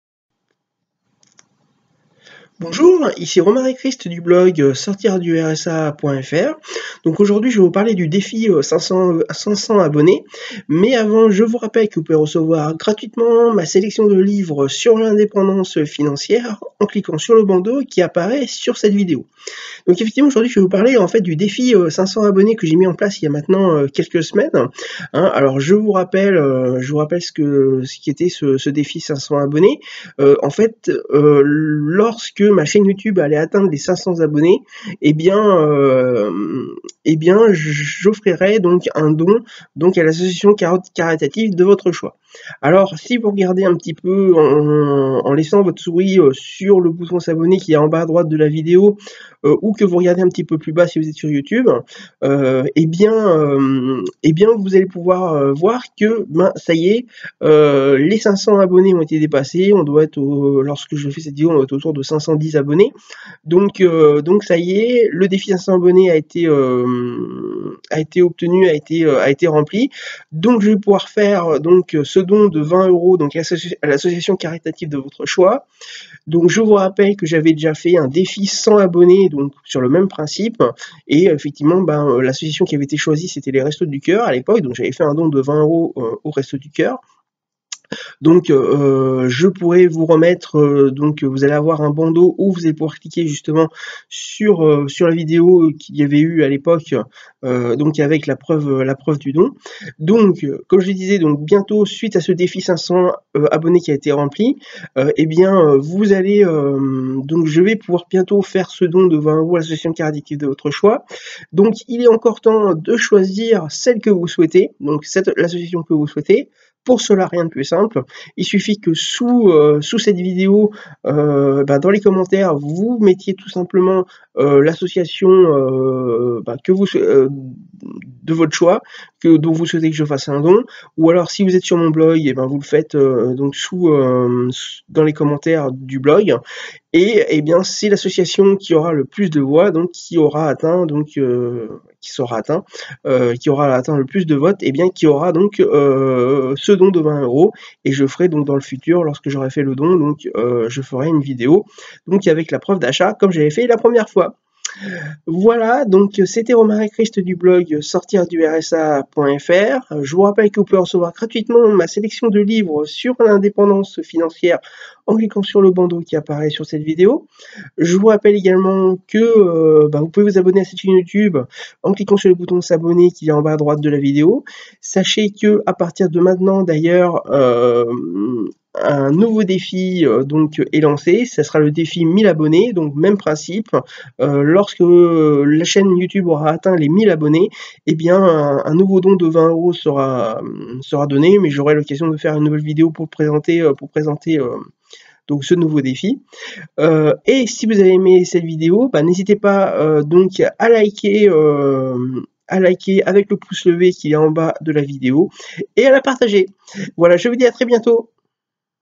défi 500 ABONNES YOUTUBE atteint ! Sortir-du-rsa.fr
Dans cette vidéo, je vous donne des nouvelles du défi 500 abonnés Youtube.